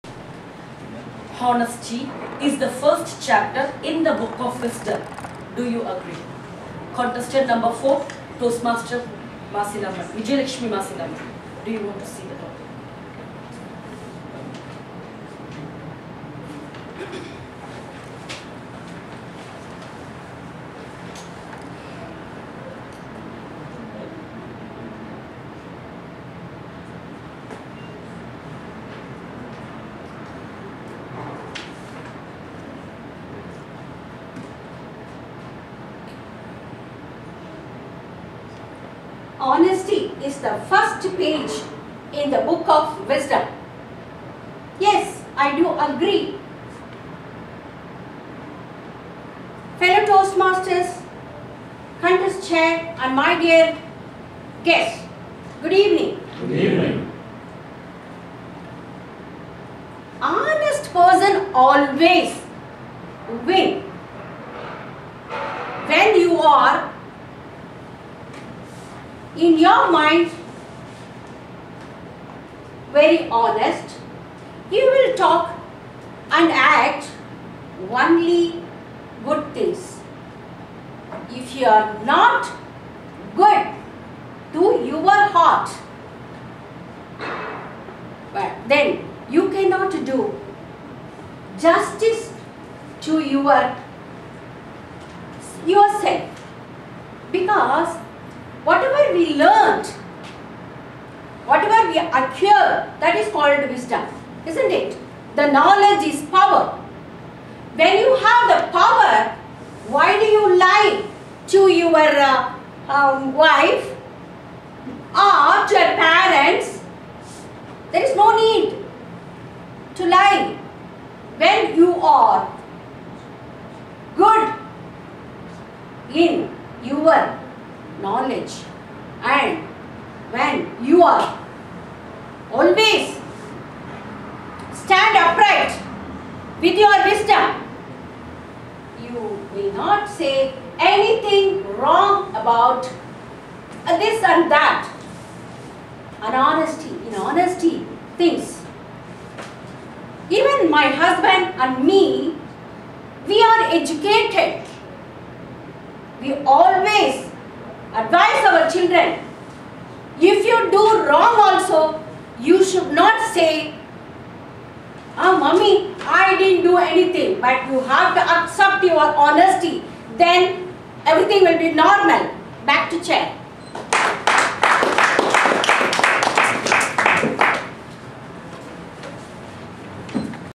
ரியாத் சொல்வேந்தர் மன்றம் : திடீர் பேச்சு : சொவே. விஜி. மாசிலாமணி
ரியாத் சொல்வேந்தர் மன்றத்தின் ஆண்டு இறுதி மன்றப் போட்டிகள்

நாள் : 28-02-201

இடம் : பாரத் ரெஸ்டாரன்ட் அரங்கம்